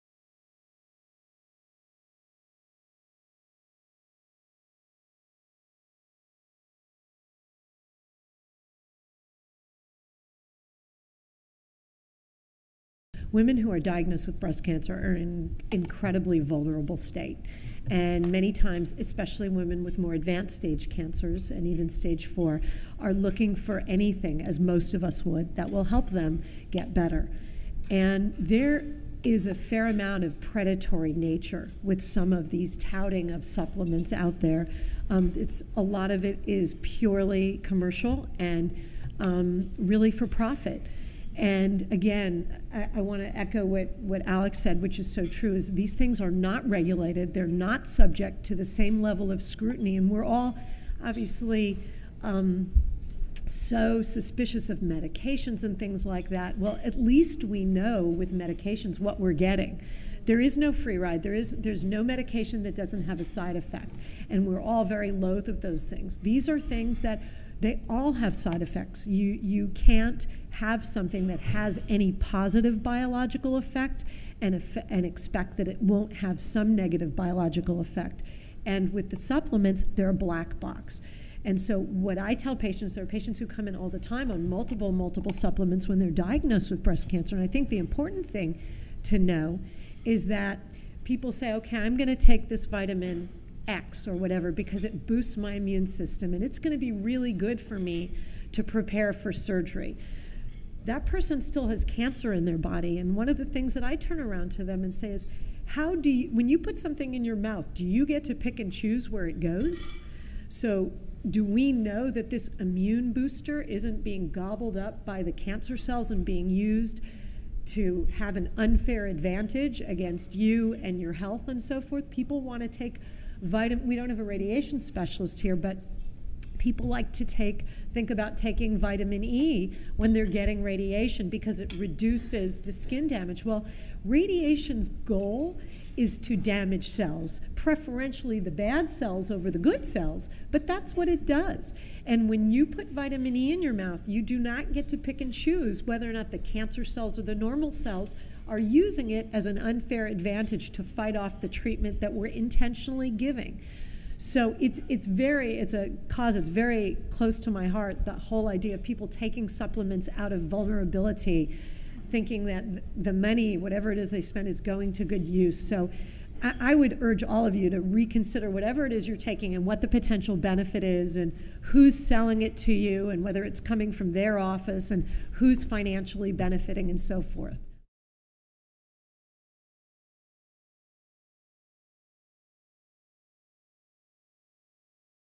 Dr. Elisa Port discusses use of Supplements during Breast Cancer Treatment
Elisa Port, MD, FACS, Co-Director of the Dubin Breast Center and Chief of Breast Surgery, discusses the use of supplements among patients undergoing breast cancer treatment, from the "Fact vs. Fiction: Everything you want to know about breast cancer, diagnosis, treatment and prevention" panel discussion held on September 20, 2012.